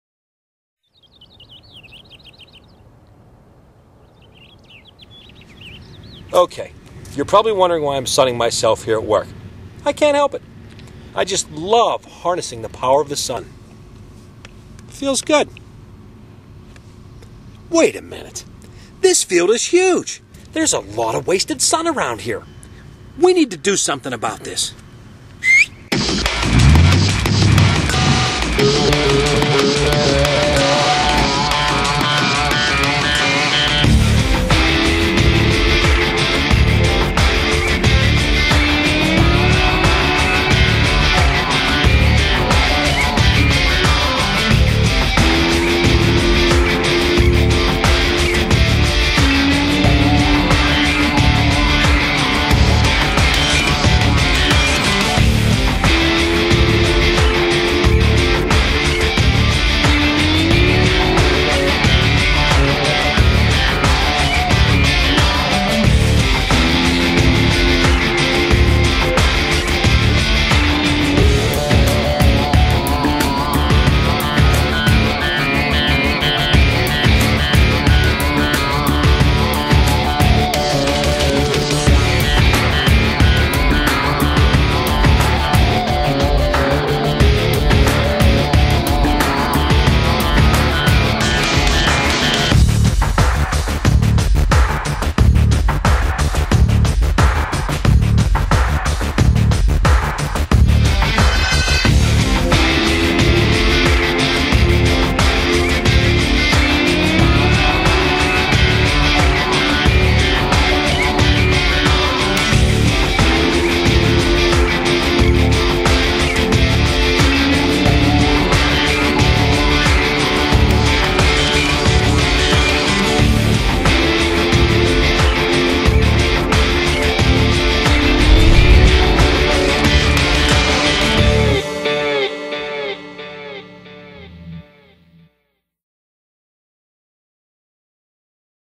GE Durham Engine Facility Solar Site
Time lapse video of the installation of the GE Durham engine facility solar site.  The site features 9072 thin film solar panels, a 700 kW Brilliance inverter, and powers 1/3 of the facility's peak load.  The solar site has been in operation since May 2011.